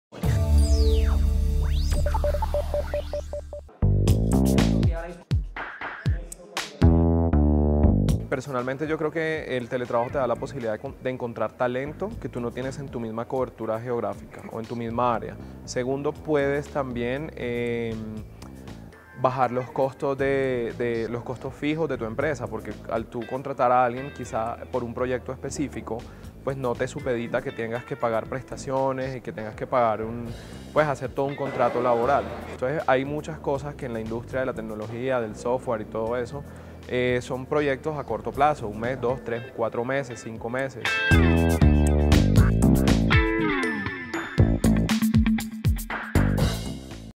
Empresas que trabajan con proyectos cortos, subcontratan teletrabajadores constantemente
Lazarillo es una empresa que genera herramientas para mejorar la calidad de vida de personas con discapacidad cognitiva. Francisco Agámez, comenta cómo su empresa utiliza el teletrabajo como una forma de operar.